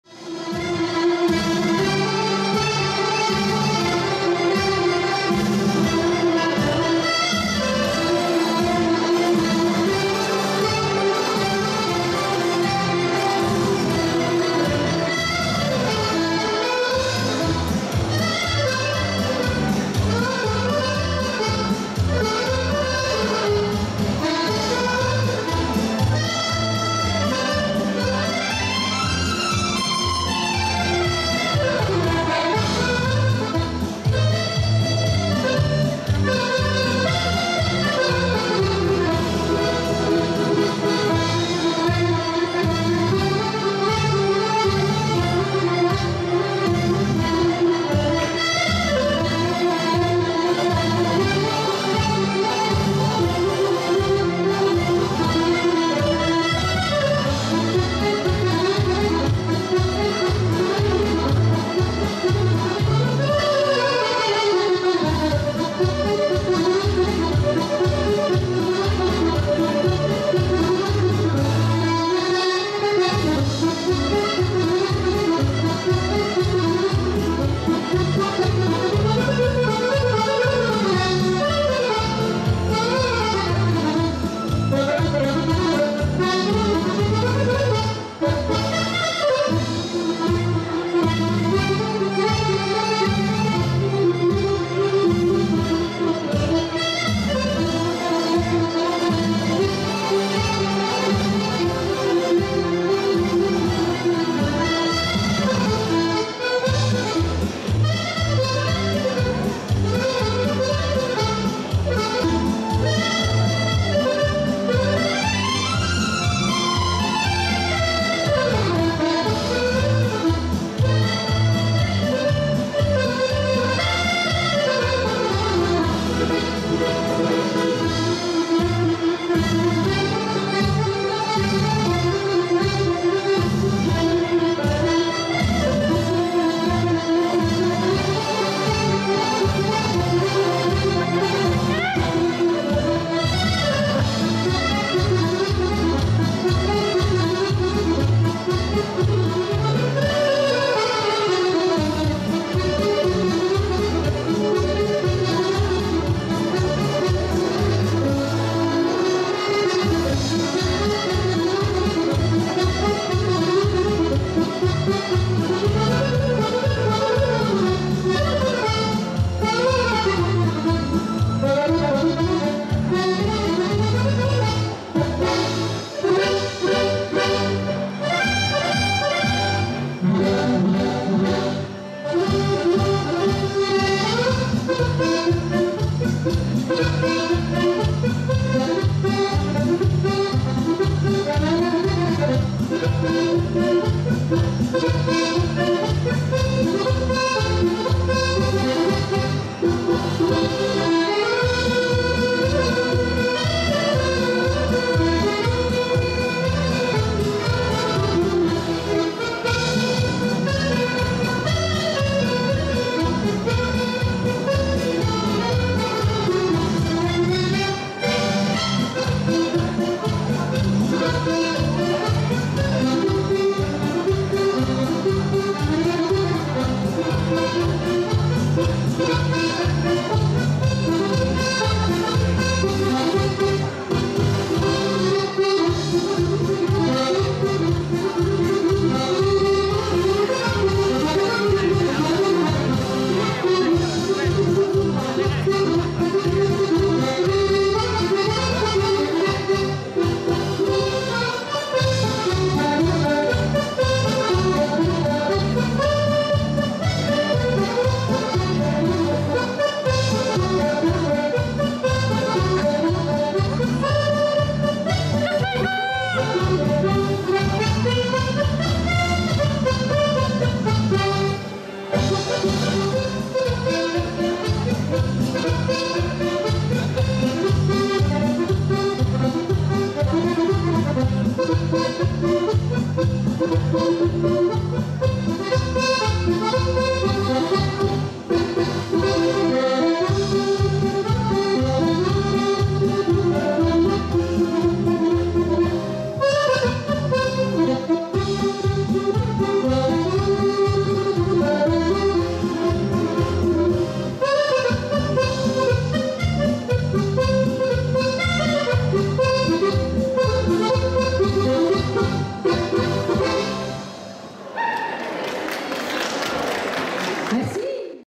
Sandrine TARAYRE Tolerme juin 2023 Pasodobles
Sandrine Tarayre et Fabien Veyriras au lac de Tolerme (46) chez Gérard et Christiane Lacaze - vidéo et réalisation Alexandra Astorg (Sandy)